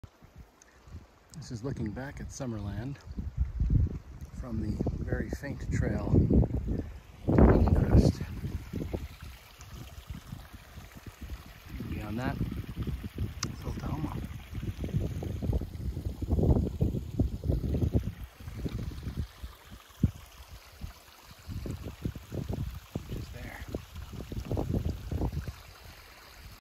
Above Summerland in Meadows
Above Summerland in Meadows of Mt Rainier NP, eyeing path upward.